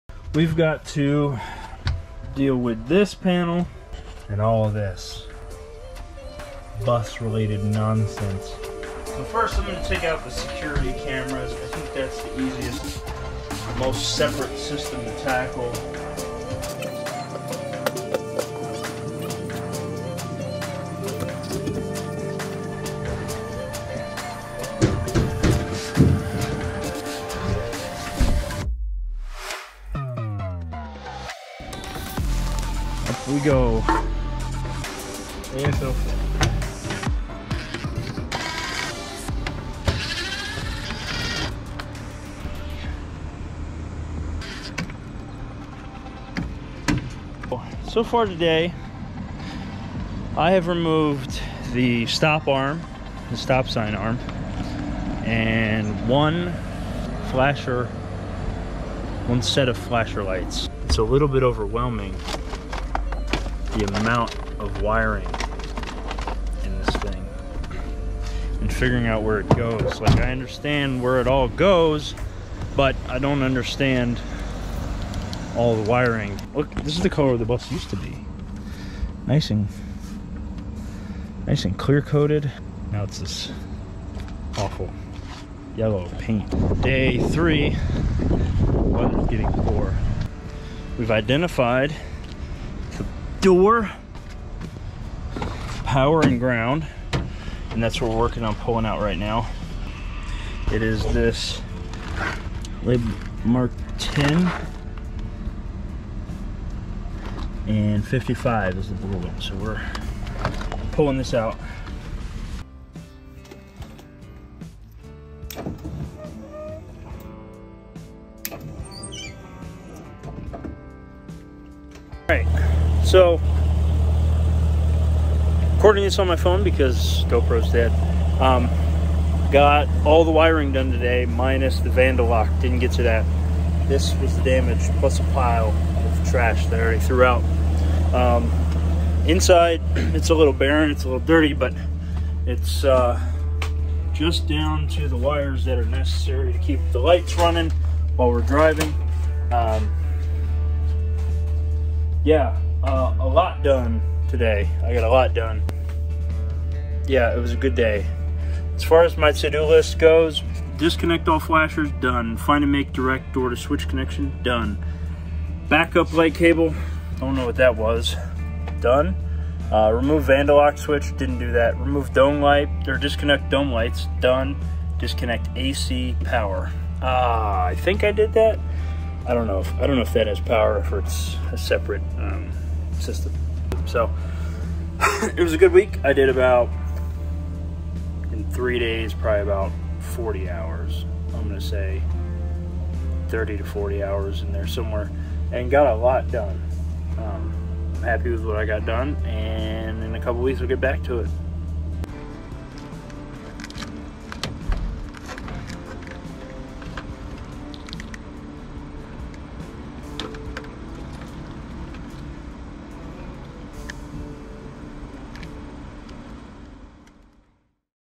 Removing Skoolie stock electrical wiring
This week we remove the stock wiring in our Collins skoolie 2003 ford E450 short bus. Follow our journey as we work to convert Berkley into our home on wheels!